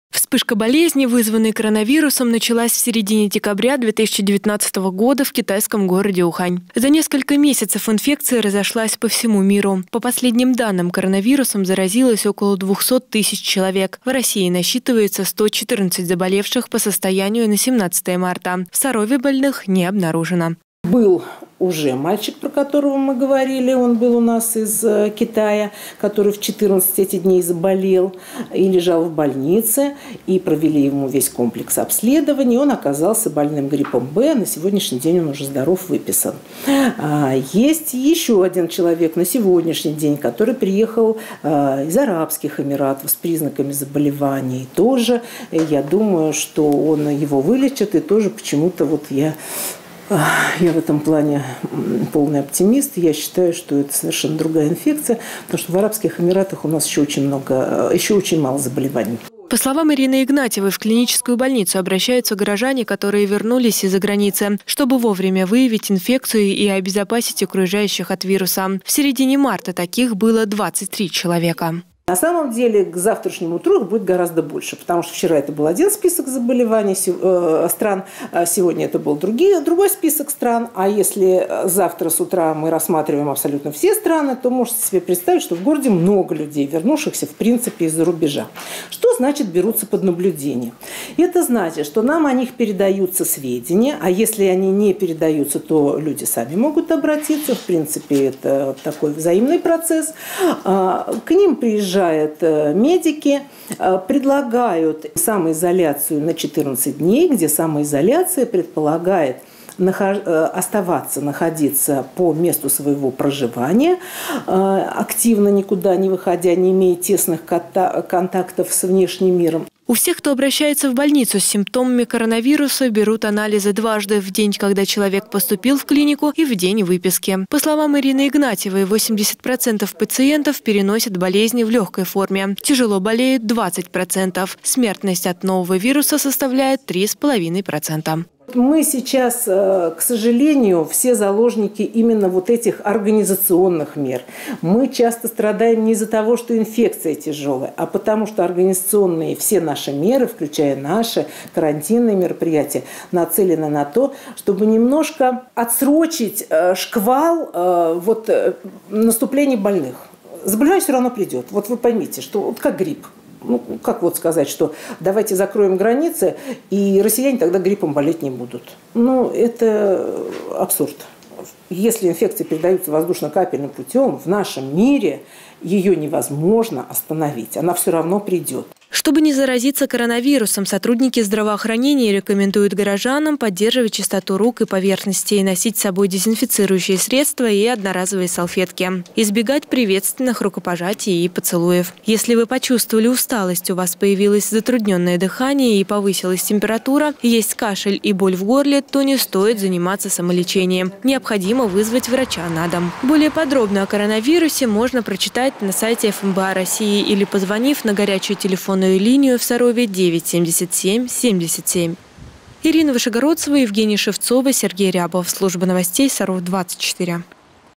Что нужно знать о коронавирусе?
Главный санитарный врач города на брифинге в администрации рассказала о ситуации по заболеваемости коронавирусом в Сарове и ответила на вопросы журналистов.